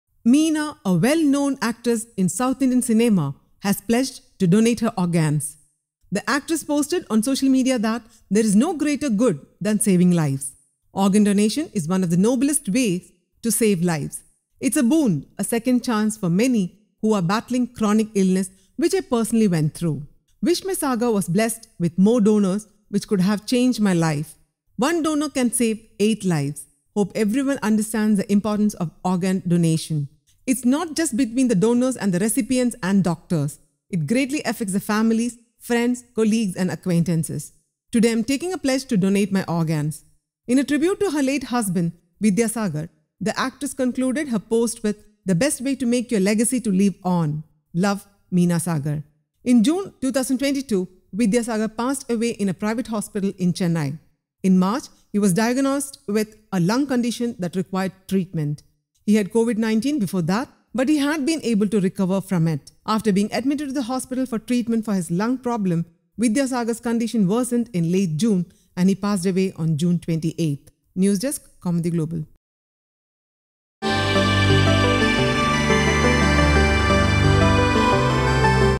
Know what Actress Meena says on her resolution for donating organs!
Meena, a well-known actress in South India, has pledged to donate her organs. The actress posted on social media that "There is no greater good than saving lives. Organ donation is one of the noblest ways to save lives. It is a boon, a second chance for many who are battling chronic illness, which I personally went through.
IANS